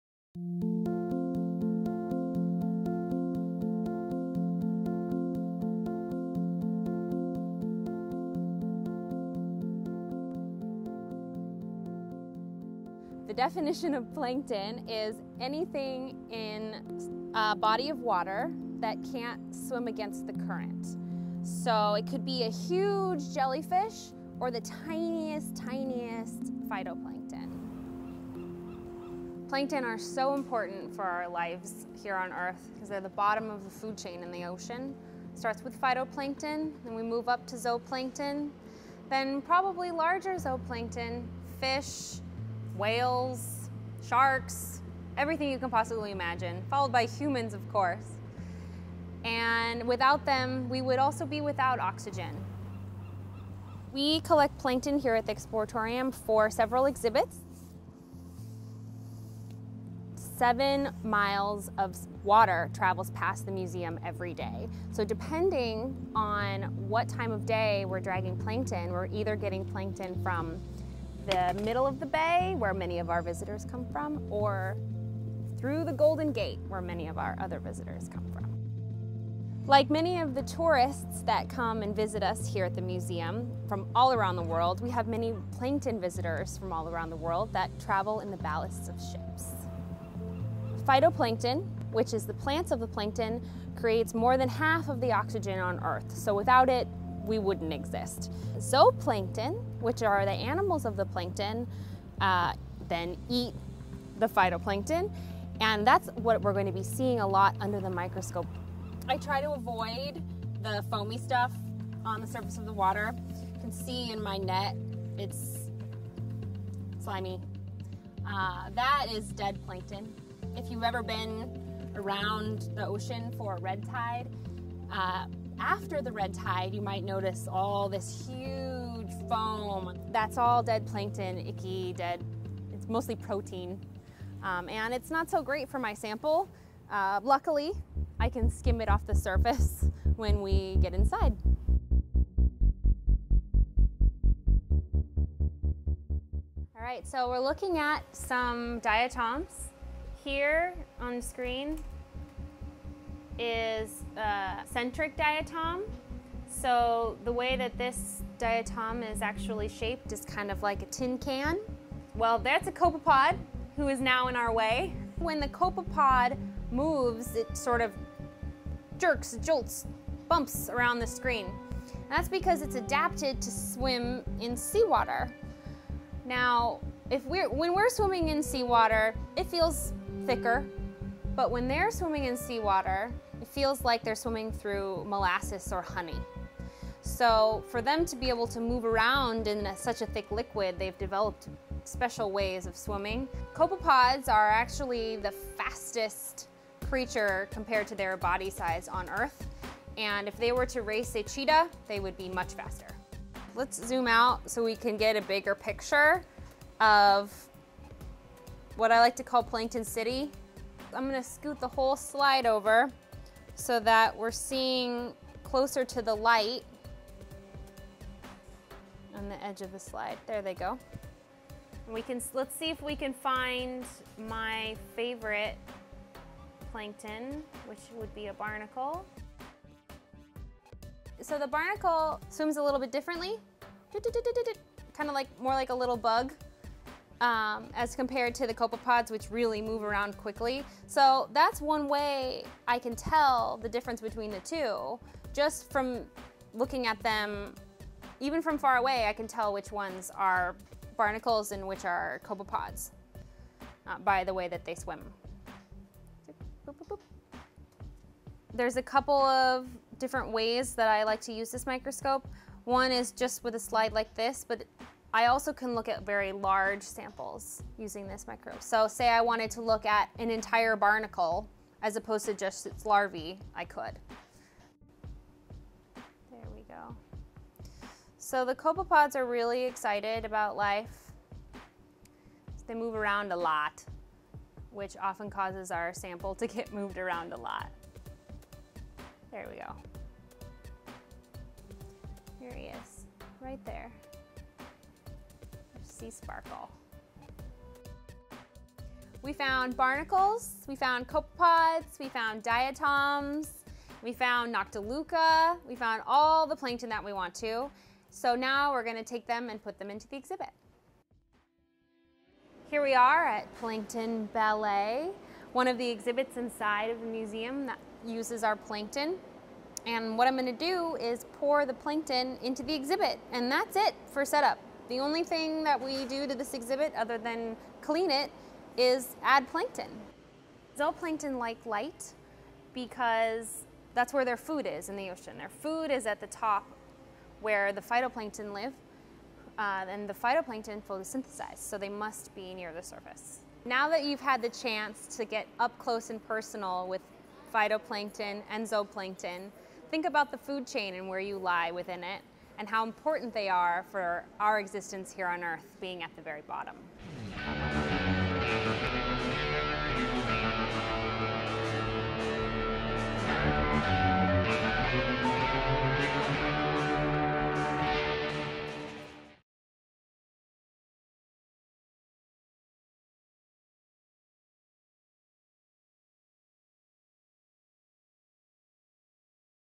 Plankton Drag I Science in the City I Exploratorium
Exploratorium biologist Karli Woodward conducts a plankton drag at our new home at Pier 15, sweeping a net through the water to survey the astonishing variety of tiny organisms that thrive in the San Francisco Bay.